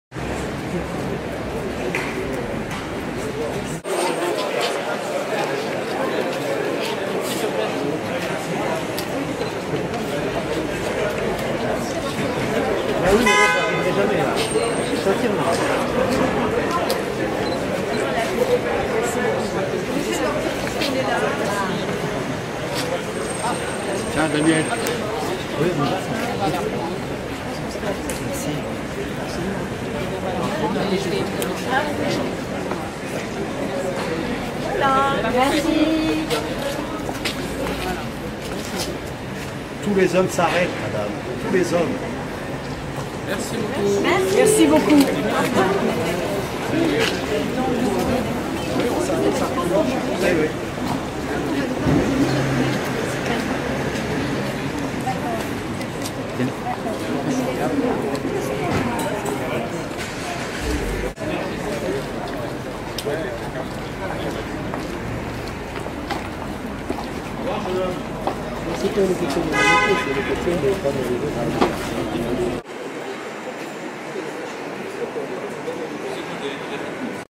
Francois Hollande Ex Wife Segolene Royal at RTL radio station in Paris
We spotted the French Politician and Ex Wife of President Francois Hollande in Paris leaving the RTL Radio Station taking pictures with fans

Paris, France on Wednesday June 12, 2013